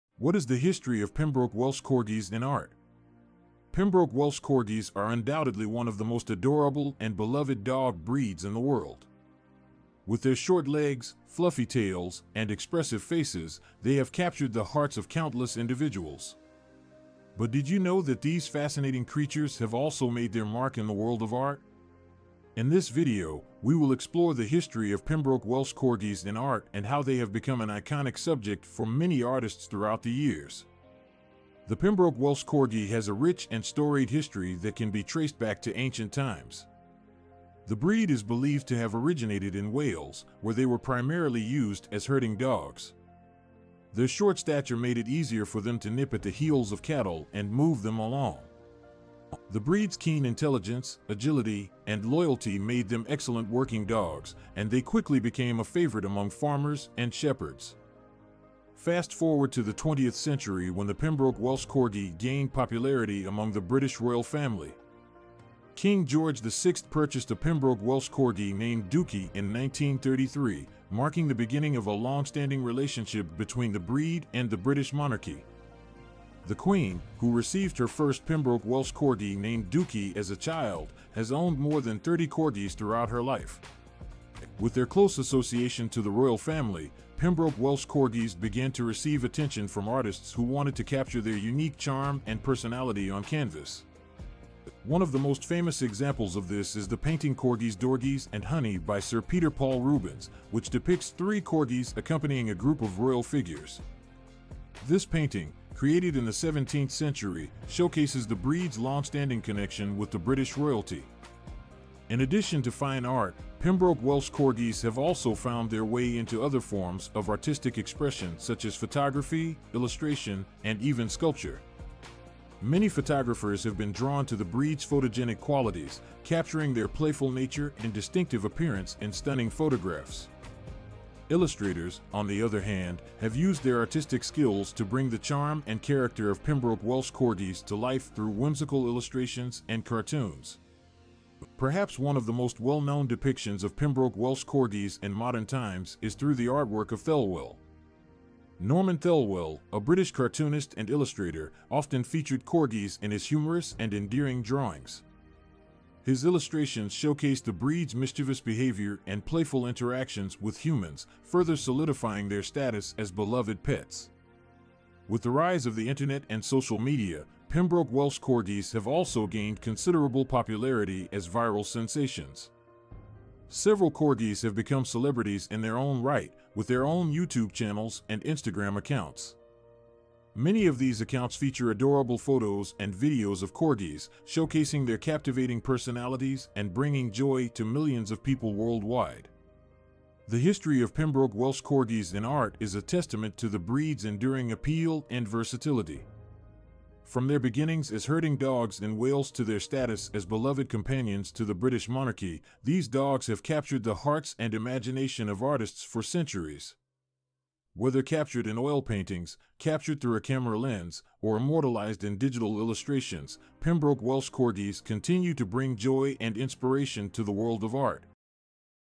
Explore the History of Pembroke Welsh Corgis in Art
Pembroke Welsh Corgis, known for their short legs, fluffy tails, and expressive faces, are one of the world's most beloved breeds of dog. Their popularity has made them an iconic subject in art, with many artists throughout the years using them as subjects in paintings, sculptures, and other artistic works. Their unique features have made them a popular choice for art that emphasizes humor and cuteness, and their small size has made them a great choice for many smaller works. Keywords: Pembroke Welsh Corgis, art, paintings, sculptures, humor, cuteness, small size.